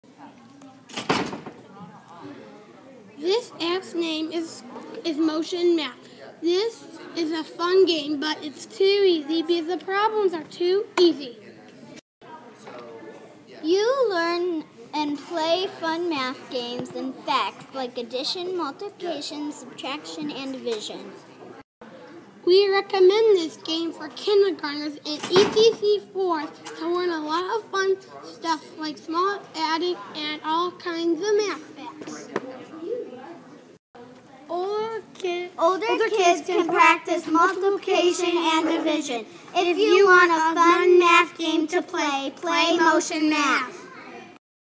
App Review: Motion Math
Third Graders at Hillel Day School worked on their writing skills by reviewing iPad apps and creating these app reviews for you!